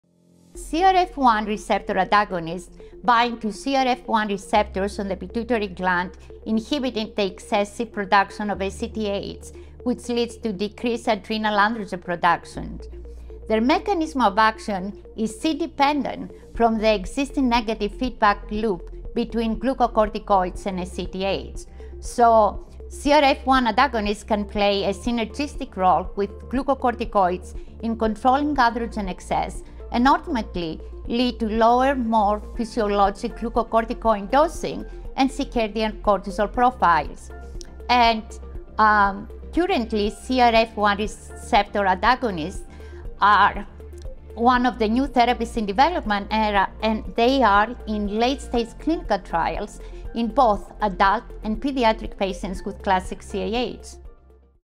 New therapy options for patients with CAH
Kiki Sarafoglou, MD, University of Minnesota Masonic Children's Hospital, discusses new therapy options for patients with CAH. Dr Sarafoglou she describes a novel mechanism of action for a newer therapy currently in late-stage clinical trials in adult and pediatric patients with CAH

Please visit The MAGIC Foundation's CAH Resource Library for additional videos on CAH